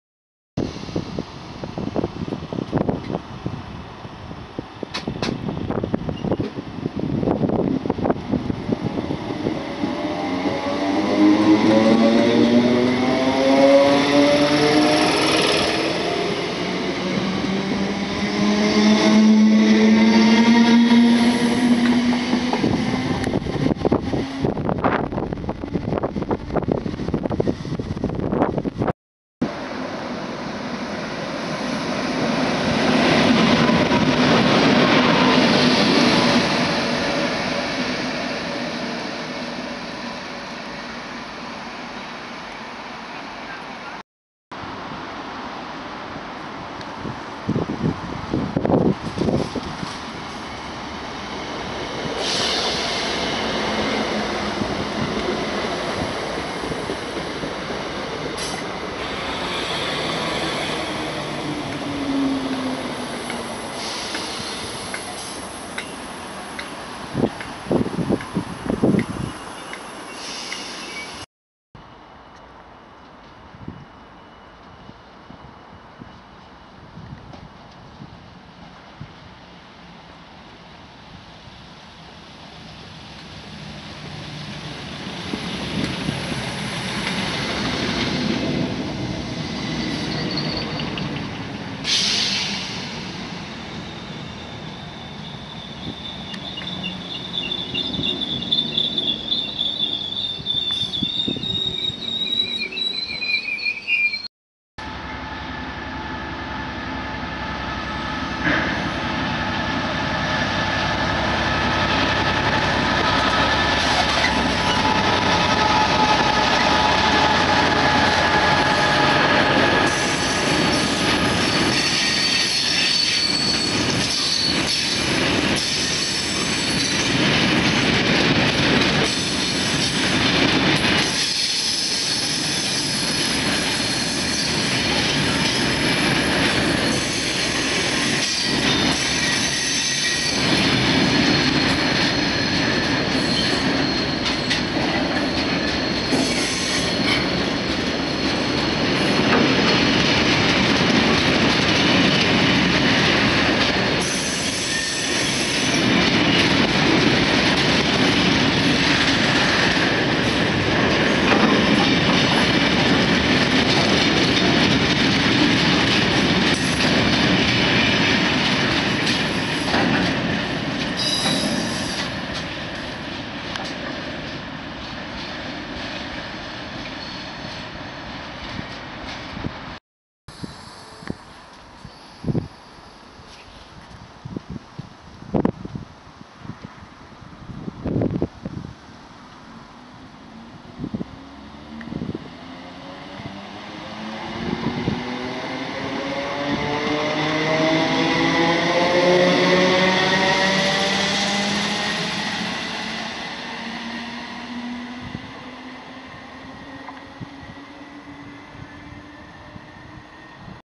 Trains at Coatbridge Central 28/08/15
Trains at Coatbridge Central 28/08/15 Not allot came by coatbridge than 320's and 318's. Enjoy!

Trains seen in this video:

Class 320 EMU

2x Class 86 Freightliner

Class 66 EWS

And Class 318 EMU

Class380Fan 2015. All videos are owned and created by me. Please respect the rules. Thanks.